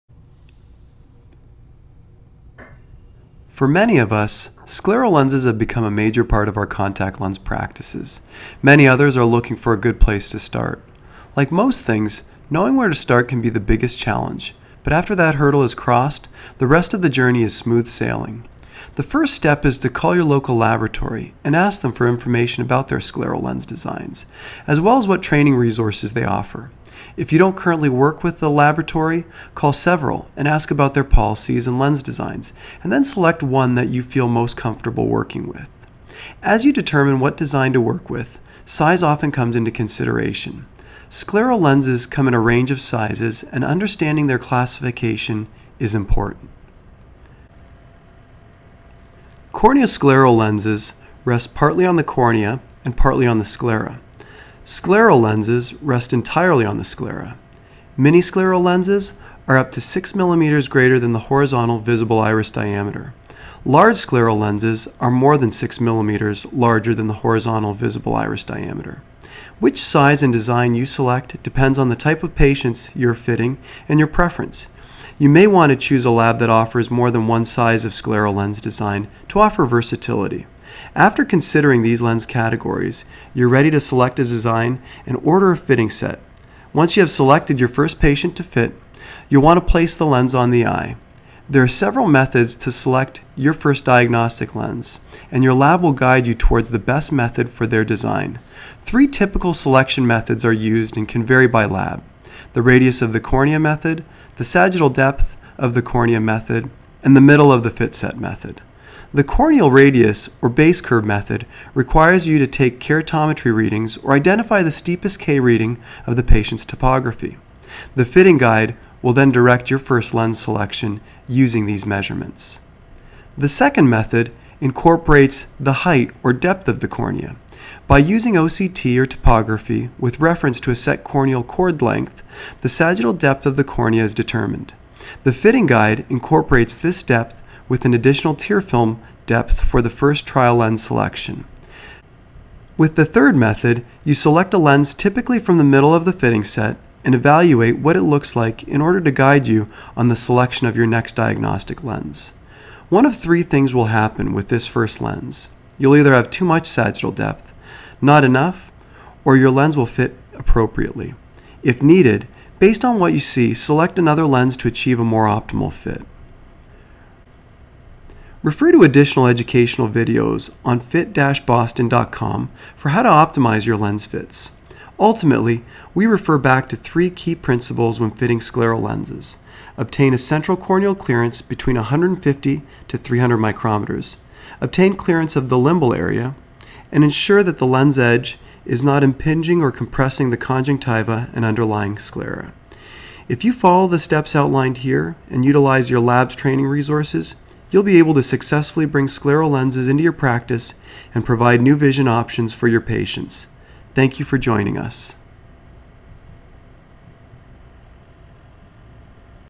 Initial Lens Selection, Ch. 5: Scleral Lens Fitting Series
Eye Care Professionals guide to Scleral Lens fitting videos. Chapter 5 - Initial Lens Selection

Scleral and semi-scleral gas permeable lens designs:
-Irregular corneas
-Post refractive surgery
-Where gas permeable lens comfort is essential
-Difficulty in adapting to gas permeable lens wear
-Where a stable lens fit is required

Make Boston gas permeable lens materials the right choice for corneal health and exceptional vision.